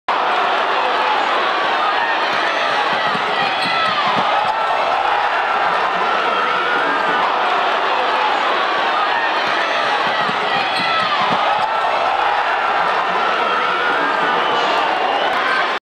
Nate Wolters at NDSU
South Dakota State point guard Nate Wolters goes behind his back for a transition layup late in the Jackrabbits' 91-88 overtime win at North Dakota State on Jan. 21, 2012.

Video courtesy of WDAY-TV in Fargo.